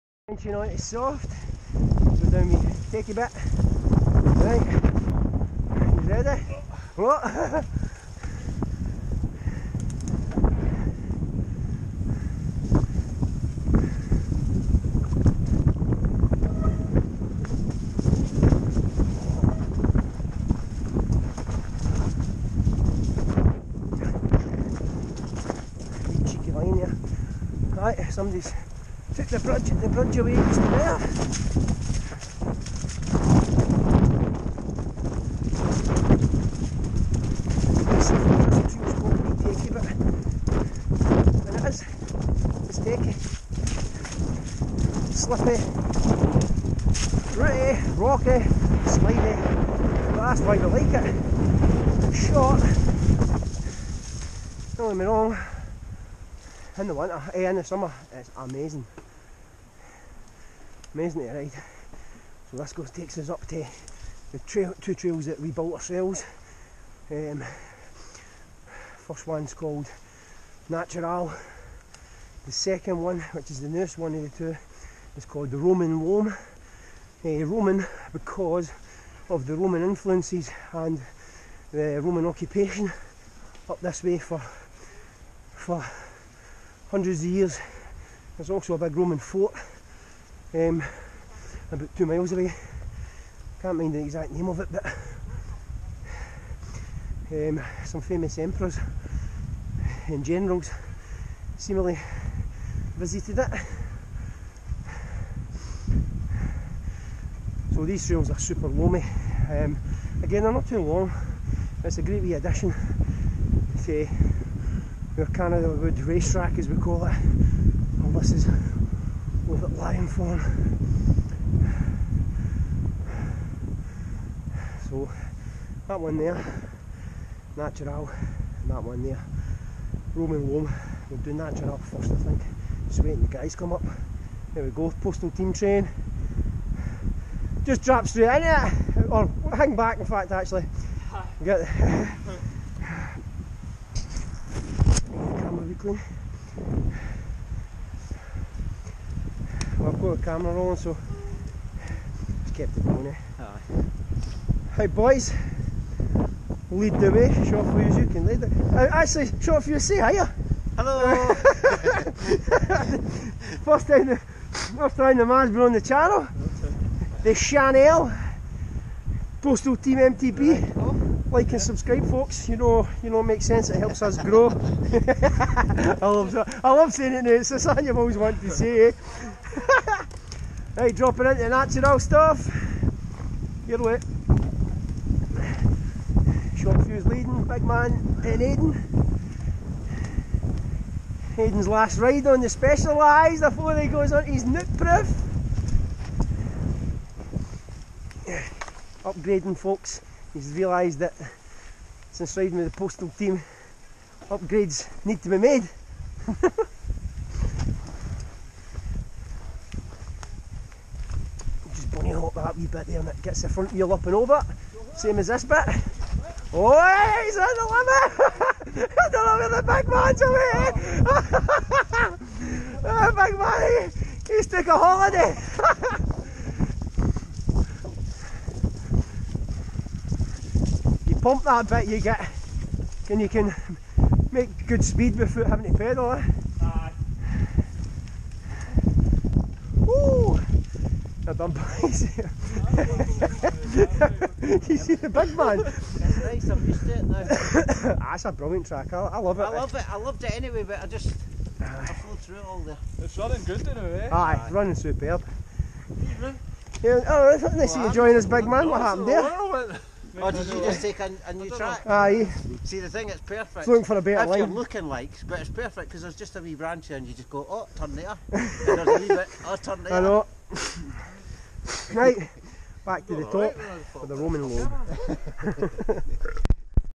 Postal team pov night ride @Canada and lion thorn woods prt3 🇨🇦🌲
Part 3 of the night tour of Canada and lion thorn woods, follow us in the pitch black as we descend down a wee techy trail called (wee techy bit) and then onto (eu natural) a natural loamy trail. just kept the camera rolling as it let's uz see the wee climb up to natural and how dark it is in the trees! look out for the big man taking a different line from the rest of us! 😬